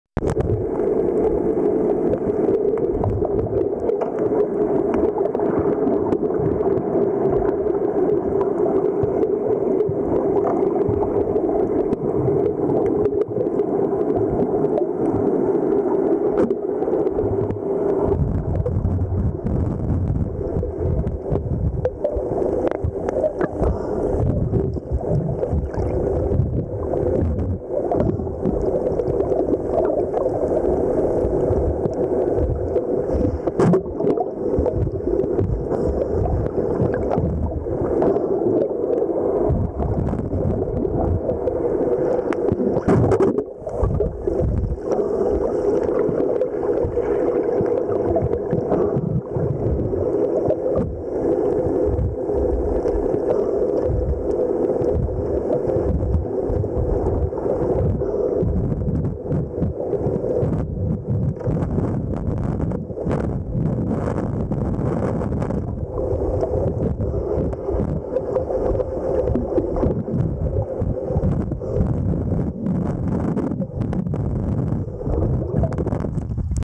к дельфинам ! PC080012
выездной снорк из Хургады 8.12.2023